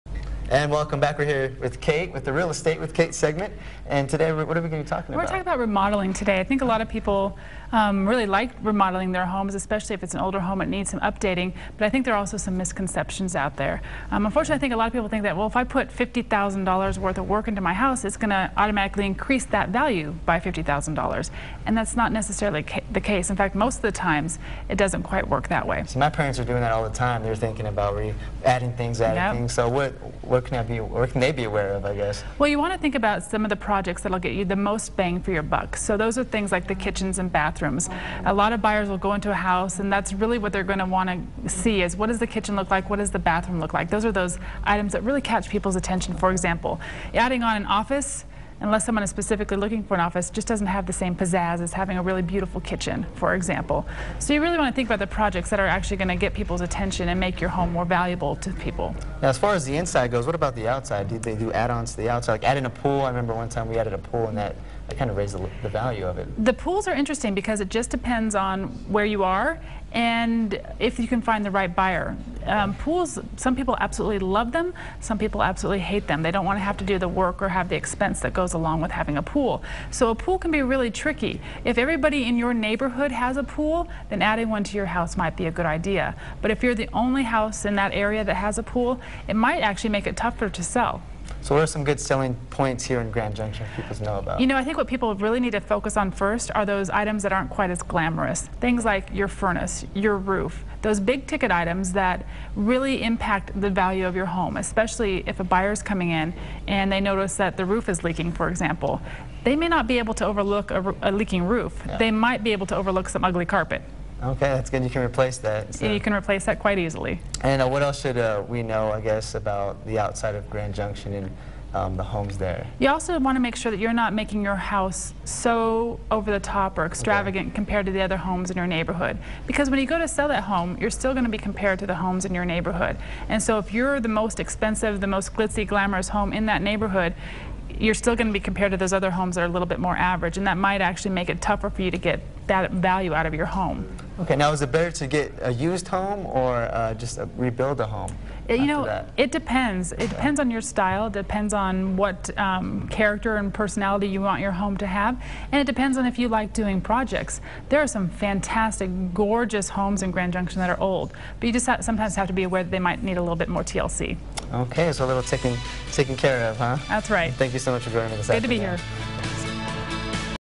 Real Estate With Kate: Home Remodeling
Kate Porras explains cost effective ways to remodel your home and what sections of the home to remodel.